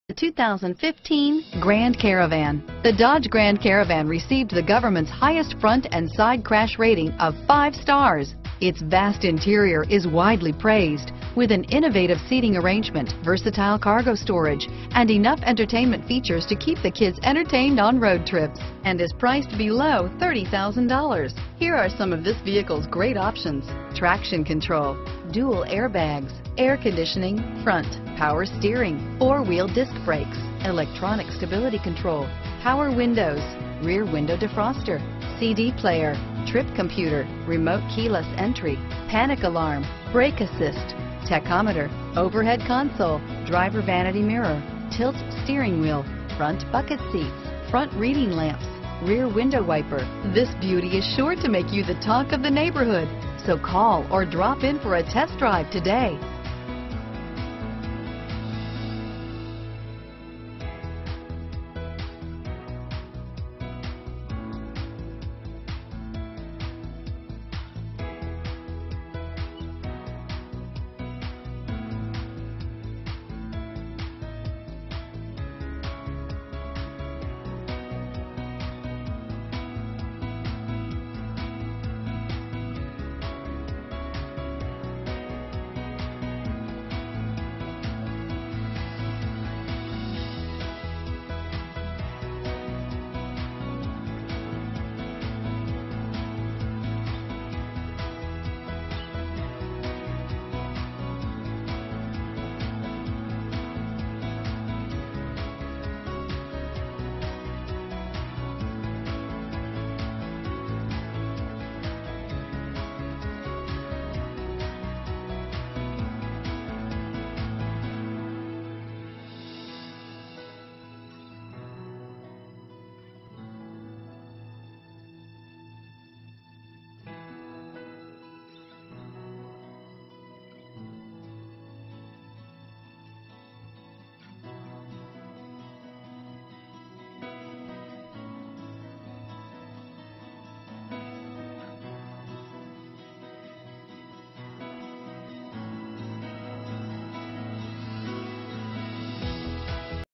2015 Dodge Grand Caravan Columbus, Hilliard, Dublin, London, Springfield, OH FR516005
2015 Dodge Grand Caravan

Performance CDJ
1130 Automall Dr.

Power Window Group (2nd Row Power Windows and Power Quarter Vented Windows), 3.6L V6 24V VVT, and 6-Speed Automatic. A real treat to drive. brbrSet down the mouse because this 2015 Dodge Grand Caravan is the van you've been searching for. This Grand Caravan is nicely equipped with features such as Power Window Group (2nd Row Power Windows and Power Quarter Vented Windows), 3.6L V6 24V VVT, and 6-Speed Automatic. Don't let the drumming of road noise wear you down. Bask in the quiet comfort of the cabin of this Dodge Grand Caravan SE. Life Time Car Wash Included!